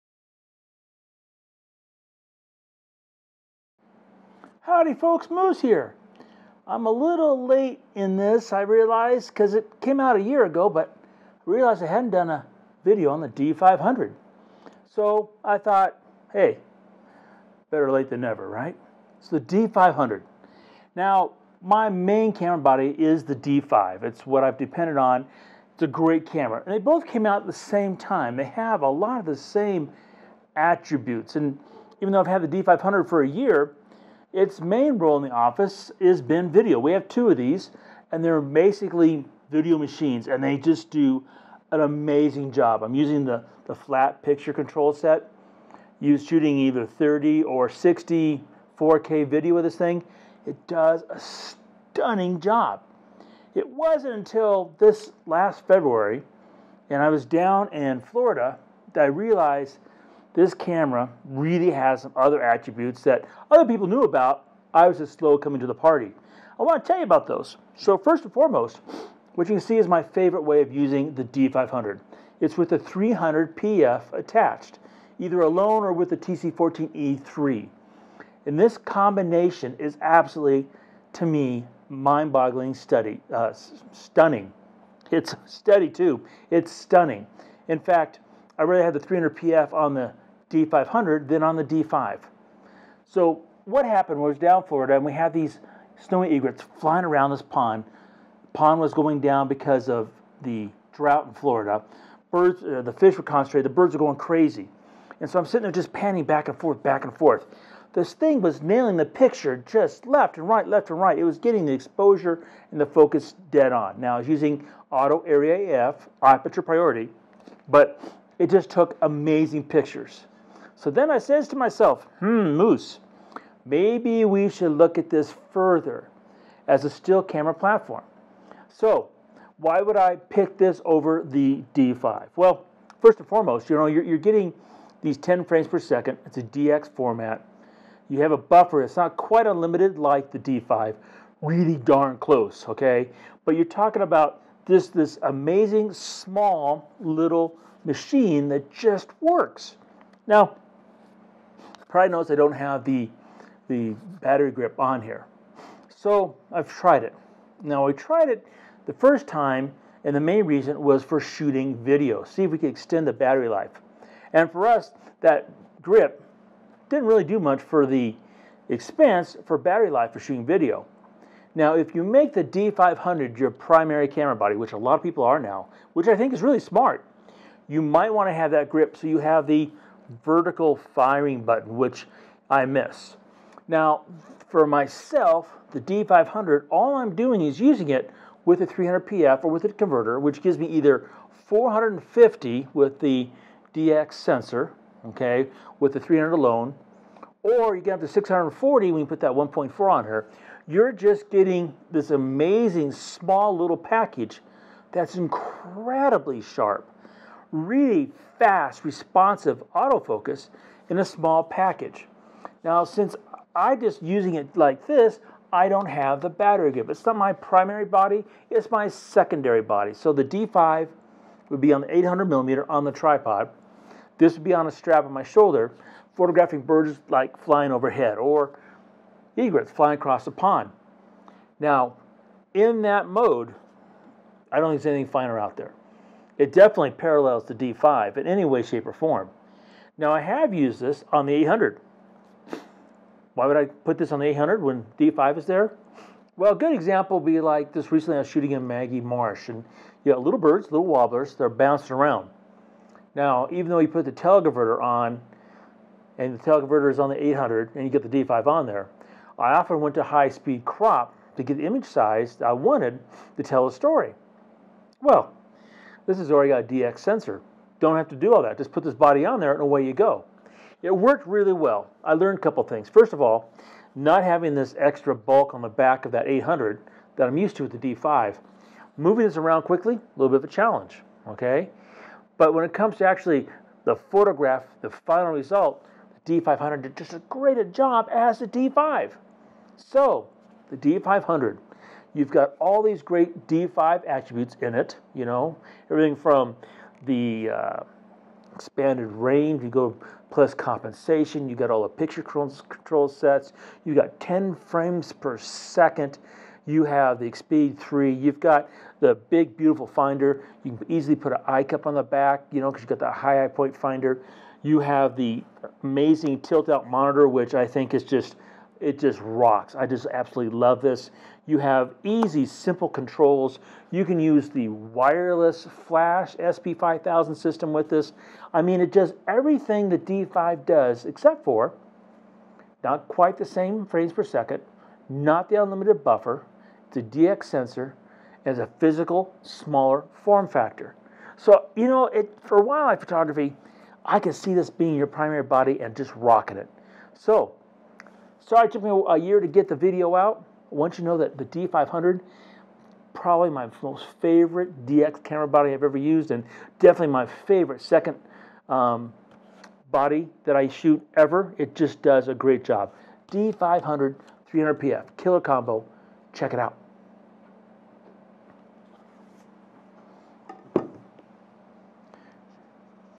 The Nikon D500 - One Year Later
This is a simple video on the Nikon D500 one year after its release. Be sure to head to the website for the full blog post.